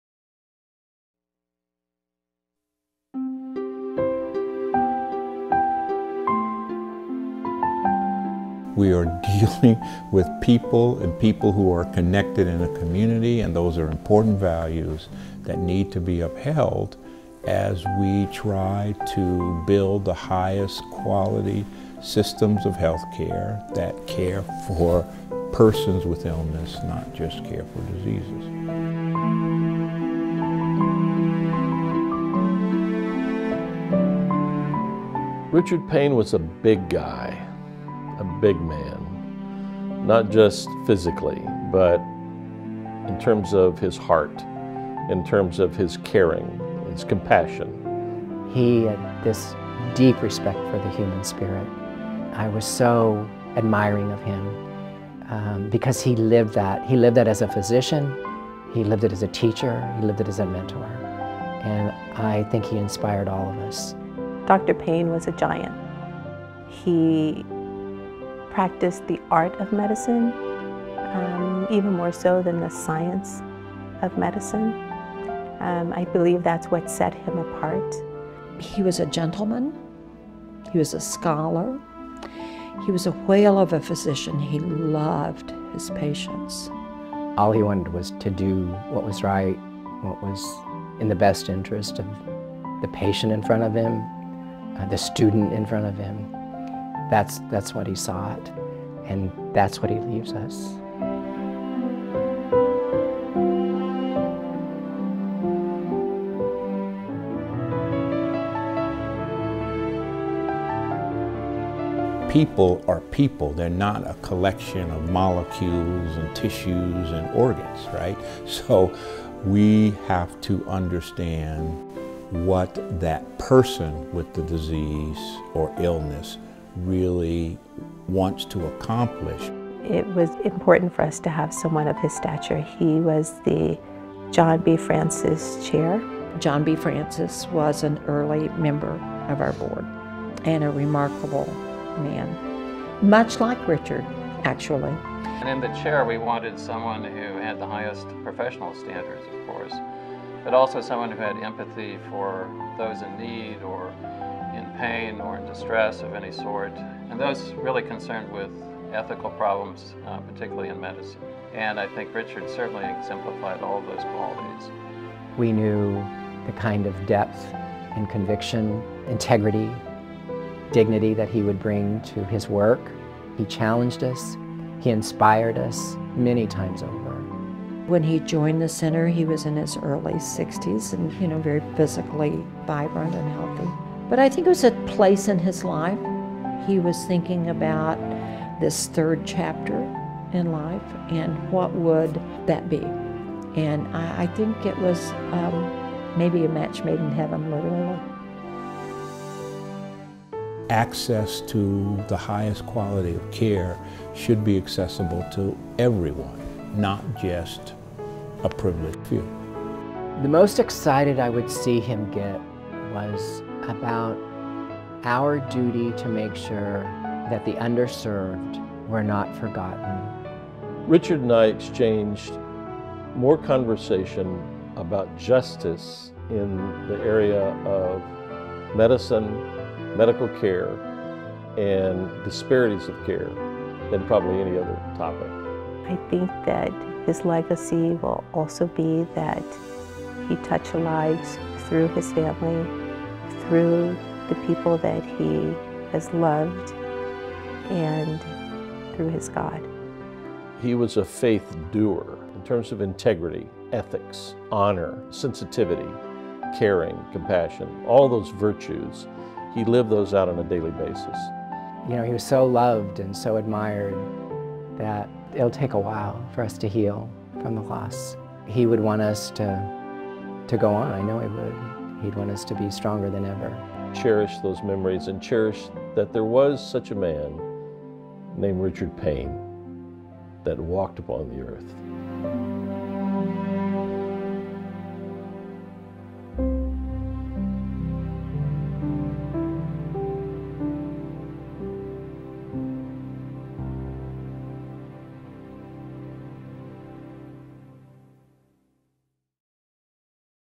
Remembering Dr. Richard Payne
The Center lost a dear colleague and friend, Dr. Richard Payne, who died on January 3, 2019, from complications related to a recent cancer diagnosis. Dr. Payne was an internationally recognized expert in neurology oncology, pain management, palliative care, care near the end of life and bioethics. He had just completed his sixth year as the Center’s John B. Francis Chair upon his death.